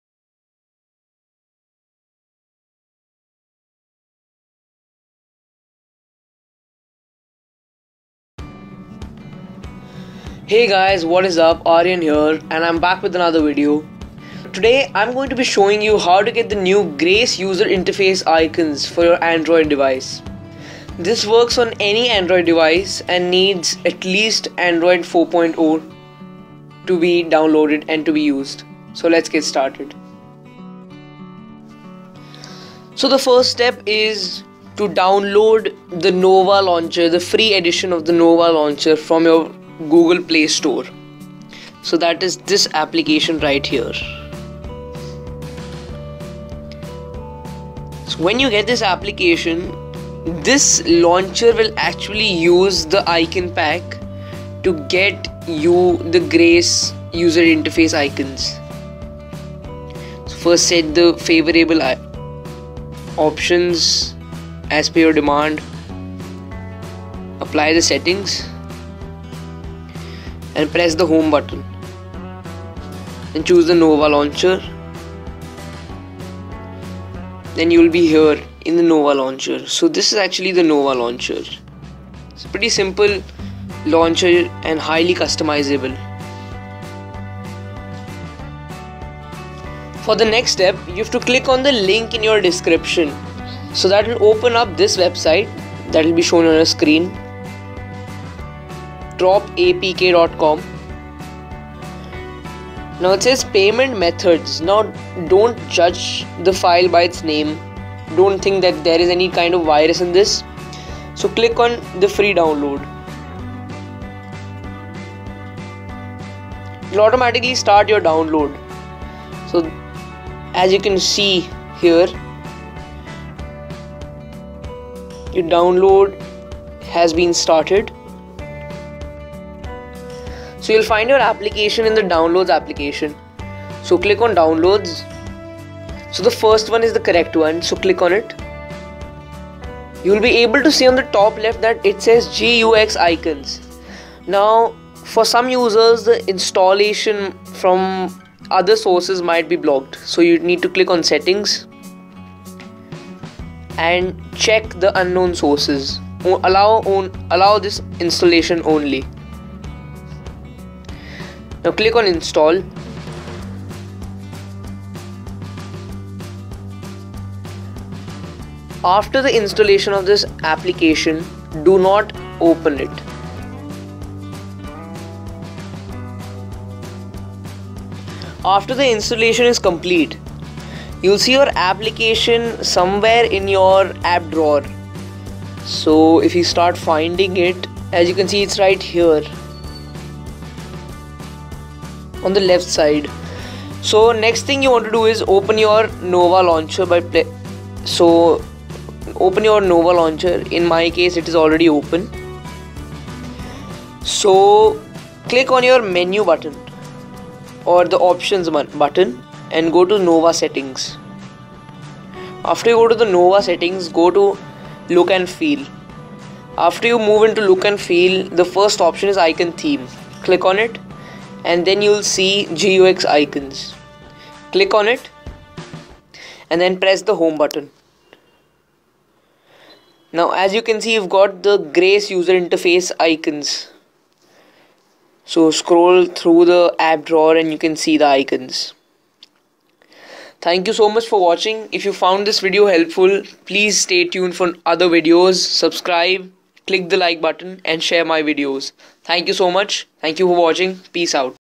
How to get the Grace Note 7 UI on any android device? | Latest version | AD Tech
Download NOVA Launcher Free edition to use.
No Virus/ No Harm

Hey guyz, this is an easy way to get your phone the new grace UI which Samsung has been and will be testing on future devices.

NOTE - Not a promotional video!

Cheers!